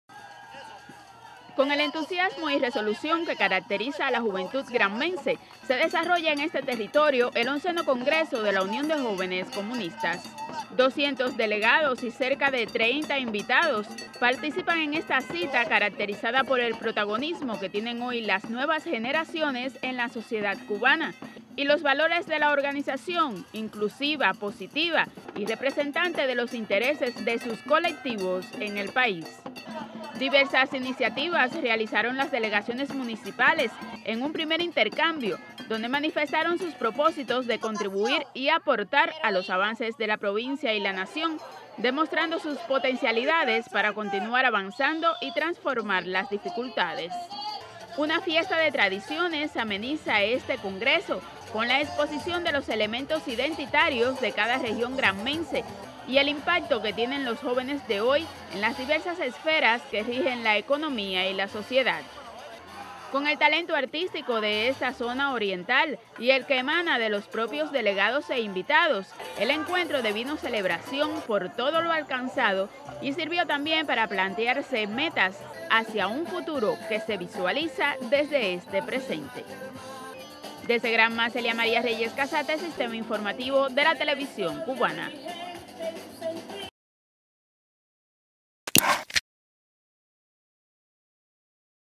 Jóvenes realizan actividades recreativas como parte del XI Congreso de la UJC en Granma
En la provincia de Granma se está desarrollando el XI Congreso de la Unión de Jóvenes Comunistas. 200 delegados y cerca de 30 invitados participan en esta cita caracterizada por el protagonismo que tienen hoy las nuevas generaciones en la sociedad cubana y los valores de la organización. Diversas actividades recreativas han ocurrido durante el evento.